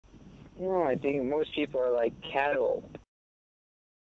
"People are like cattle" - Chris Lopez (Las Vegas New Mexico Mayor Candidate 2016)
A Freemason says "People are like cattle"